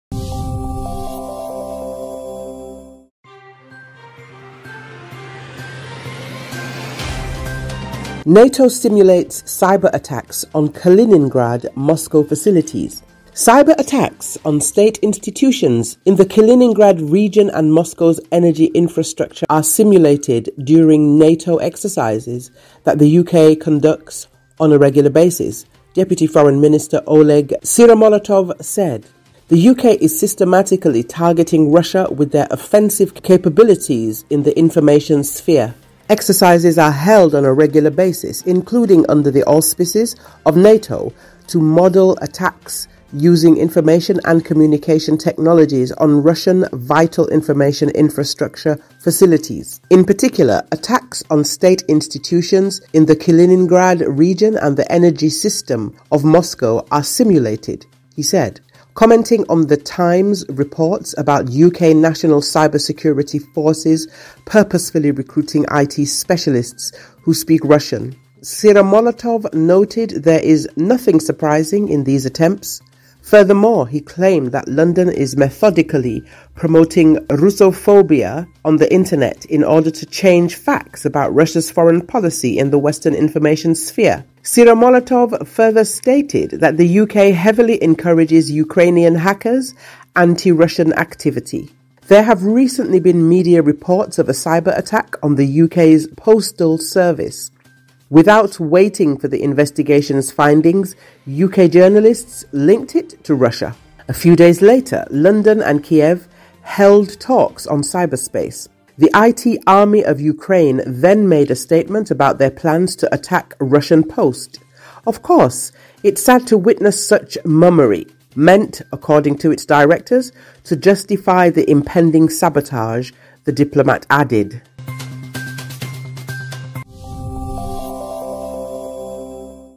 NATO simulates cyberattacks on Kaliningrad, Moscow facilities
Rusiya xarici işlər nazirinin müavini Oleq Syromolotov bildirib ki, Kalininqrad vilayətində dövlət qurumlarına və Moskvanın enerji infrastrukturuna kiberhücumlar Böyük Britaniyanın müntəzəm olaraq keçirdiyi NATO təlimləri zamanı məşq edilir.
"Böyük Britaniya informasiya sahəsindəki hücum imkanları ilə sistematik olaraq Rusiyanı hədəfə alır. Rusiyanın mühüm informasiya infrastruktur obyektlərinə informasiya-kommunikasiya texnologiyalarından istifadə etməklə hücumları modelləşdirmək üçün müntəzəm olaraq, o cümlədən NATO-nun himayəsi altında təlimlər keçirilir. Xüsusilə, hücumlarda Kalininqrad vilayətinin dövlət qurumları və Moskvanın enerji sistemi imitasiya edilib”, - deyə o bildirib.
Böyük Britaniyanın milli kibertəhlükəsizlik qüvvələrinin məqsədyönlü şəkildə rus dilini bilən İT mütəxəssislərini işə cəlb etməsi ilə bağlı “The Times” qəzetinin xəbərlərini şərh edən Syromolotov qeyd edib ki, bu cəhdlərdə “təəccüblü heç nə yoxdur”.
Bundan başqa, o, iddia edib ki, London Rusiyanın Qərb informasiya sferasında xarici siyasəti ilə bağlı faktları dəyişdirmək üçün internetdə rusofobiyanı metodik şəkildə təbliğ edir. Syromolotov daha sonra bildirib ki, Böyük Britaniya ukraynalı hakerlərin Rusiya əleyhinə fəaliyyətini ciddi şəkildə təşviq edir.